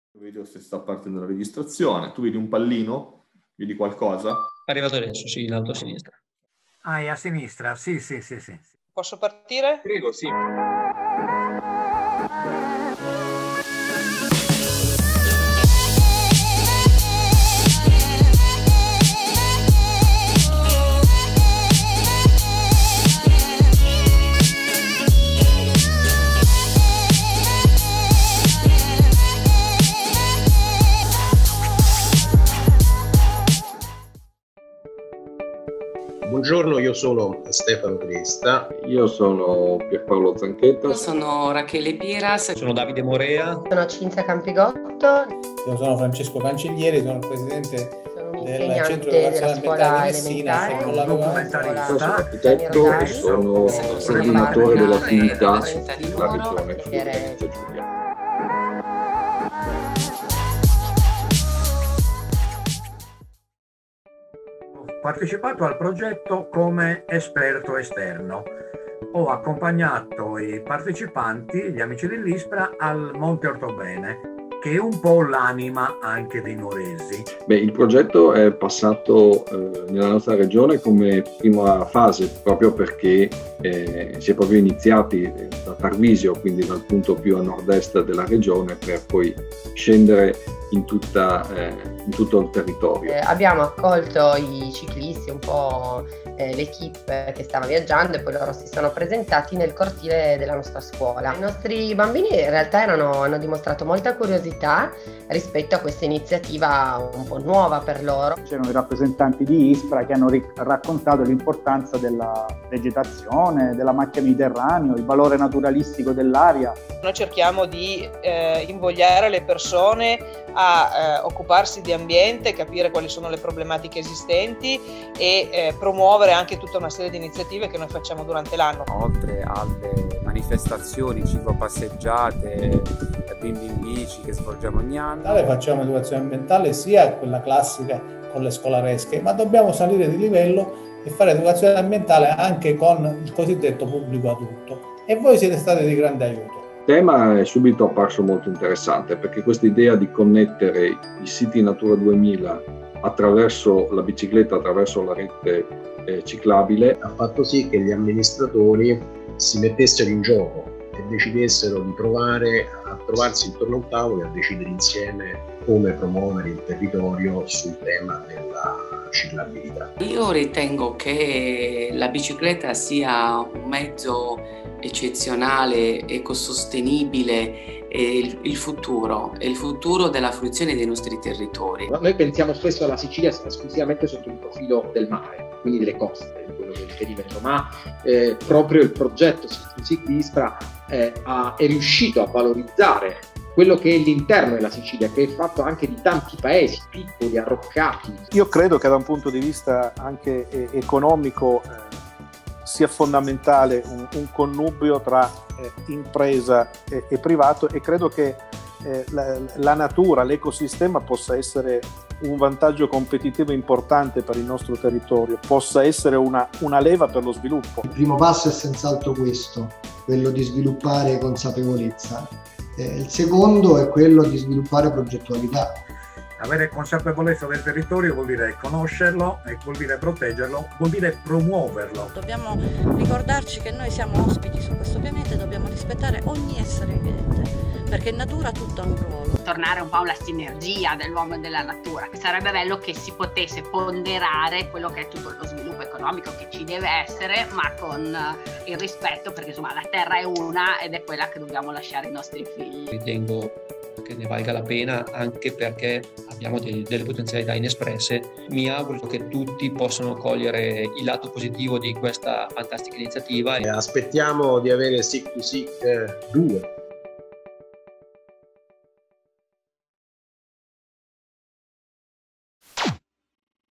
PerchèSIC
Perchè SIc2Sic? In occasione dell'evento finale del progetto Life Sic2Sic il territorio che partecipa si racconta.